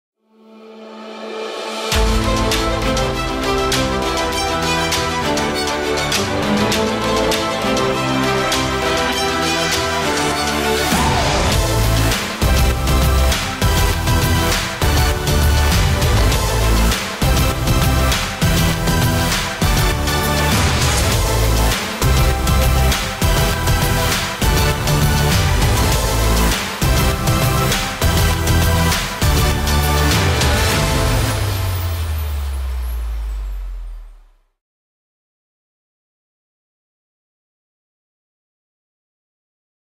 Customer Acqusition Trailer
Bronze Senior Vice President Lisa Douglass-Doe
teaches the mechanics of entering customers through
the 5linx.net website.